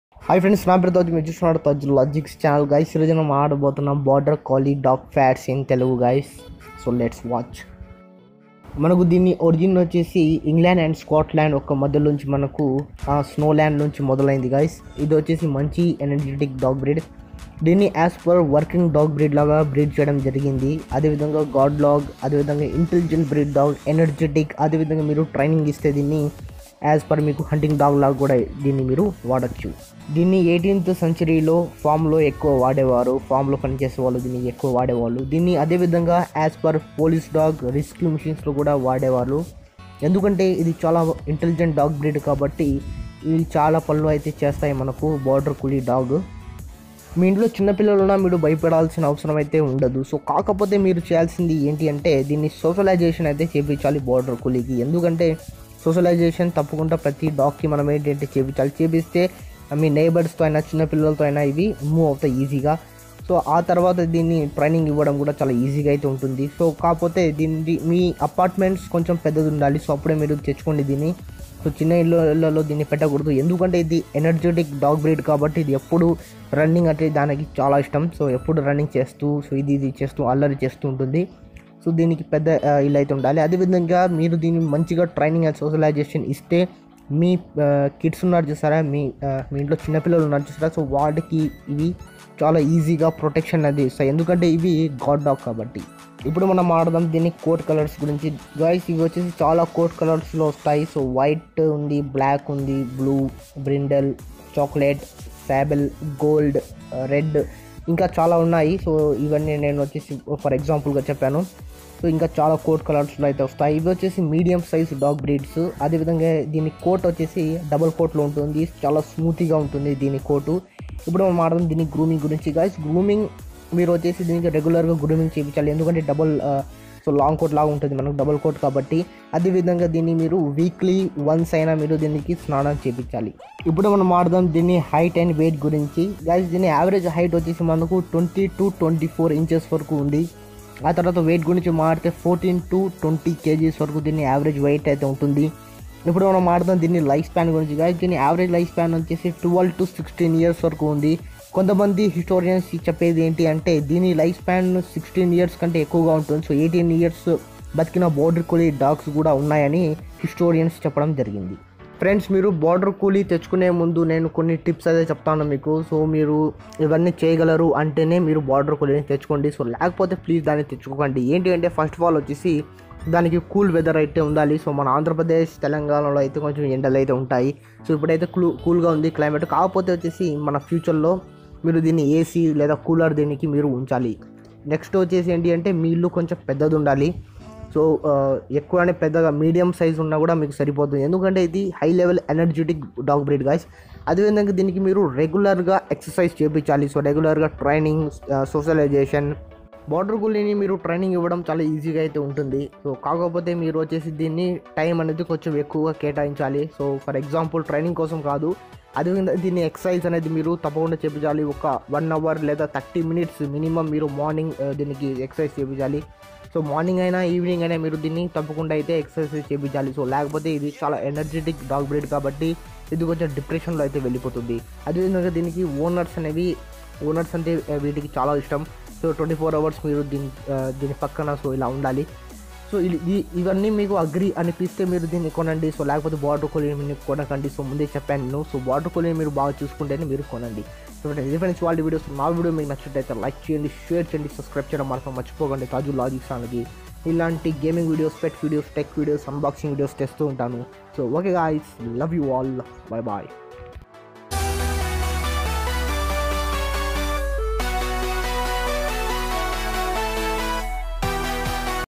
Border Collie Dog Facts | Popular dog breed | Telugu | Taju logics
The border collie is a working and herding dog breed developed in the Scottish borders for herding livestock, especially sheep. It was specifically bred for intelligence and obedience.

Colors: Black, White, Blue, Red Merle, Lilac, Brindle, Chocolate, Liver, Blue Merle.

Temperament: Tenacious, Intelligent, Energetic, Responsive, Keen, Alert